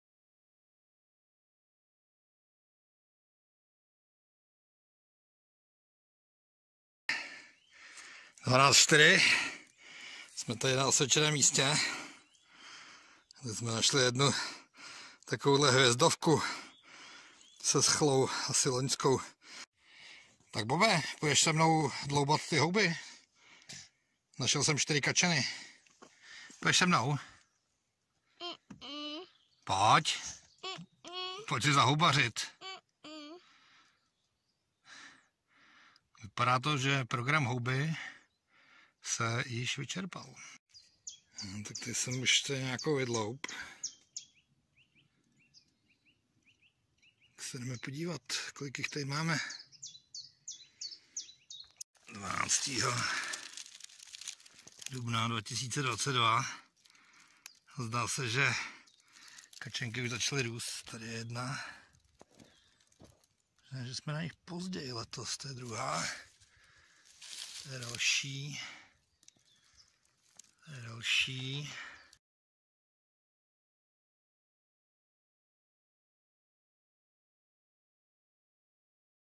Kačenka česká, Místo X, 12.4.2022
Šestá sezona sběru jarních hub s JayKayem.